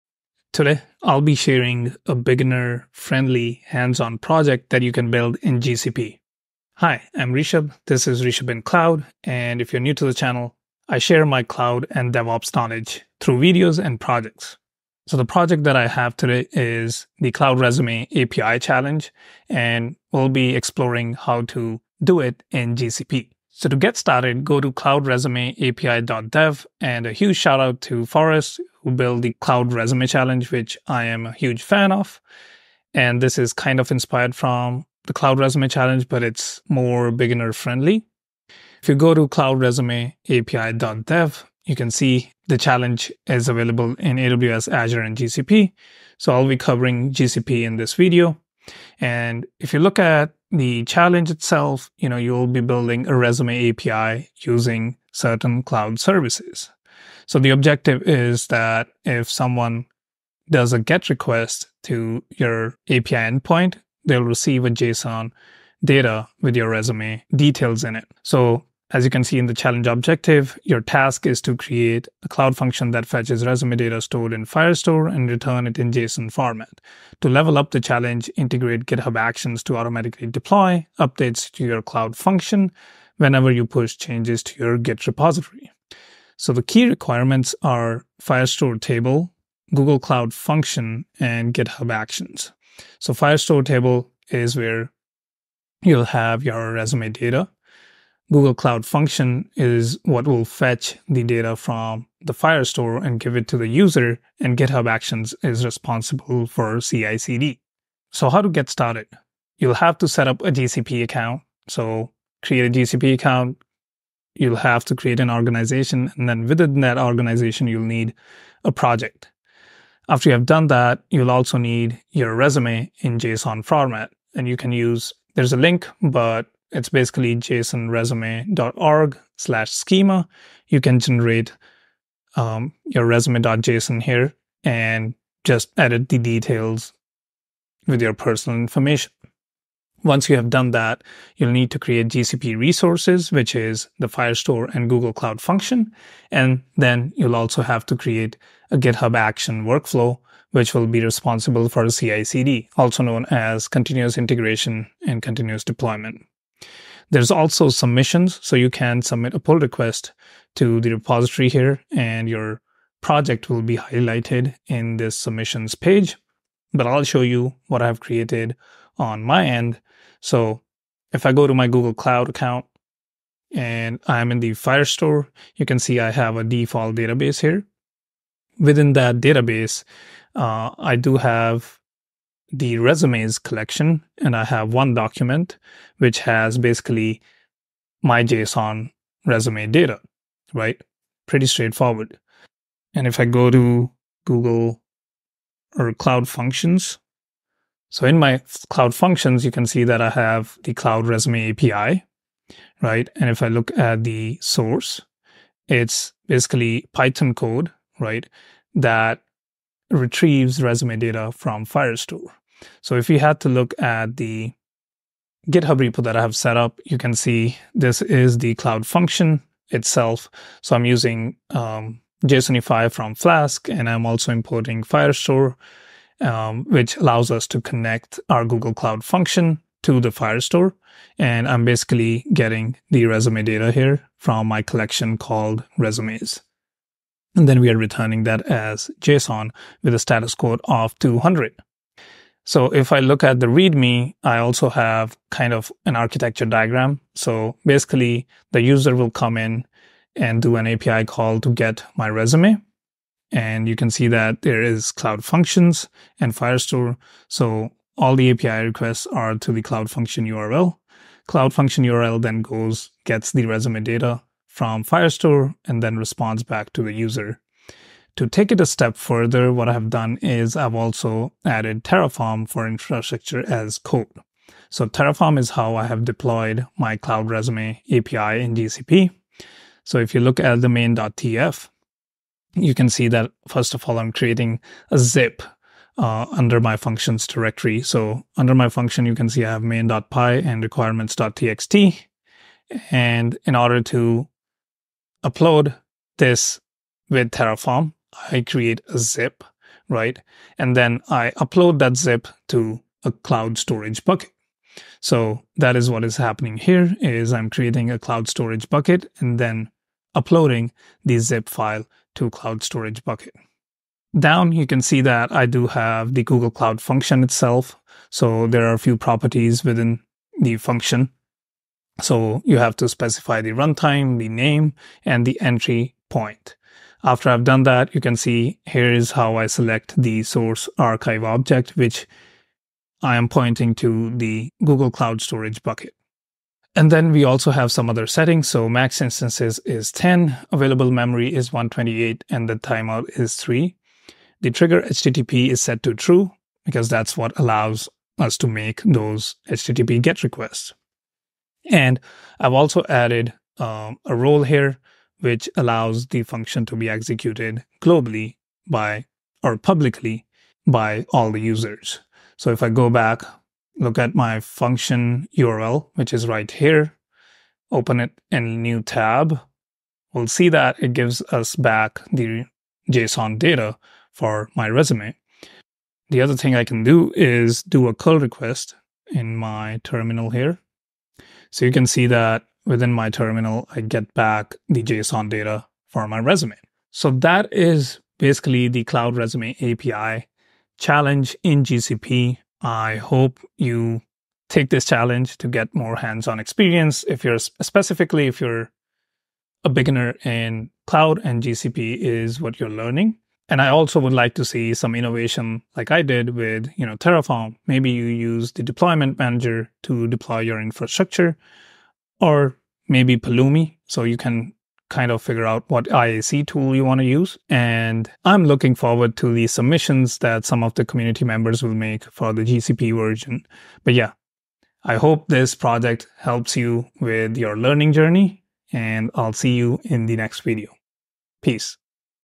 Beginner GCP Cloud Project | GCP Cloud Resume API
GCP Cloud Resume API - A project where you will build and deploy a serverless API using Google Cloud Functions and Firestore, integrated with GitHub Actions. The primary goal? Construct an API that can serve resume data in JSON format.

Timestamps:
0:00 intro
0:23 what is the Cloud Resume API Challenge
2:51 project walkthrough in Google Cloud
4:28 take it a step further with IaC - Terraform
6:04 demo of the project